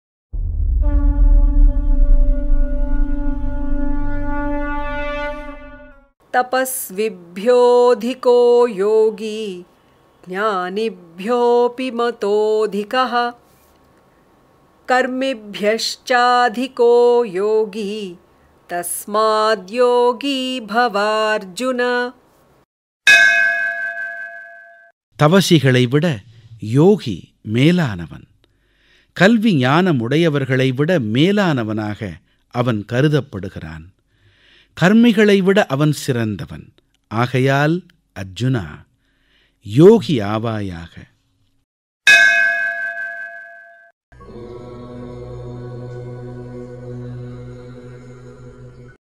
த்யான யோகம் - 46
தவசிகளைவிட யோகி மேலானவன்; கல்வி ஞானமுடையவர்களைவிட மேலானவனாக அவன் கருதப்படுகிறான். கர்மிகளைவிட அவன் சிறந்தவன். ஆகையால் அர்ஜுனா, யோகியாவாயாக.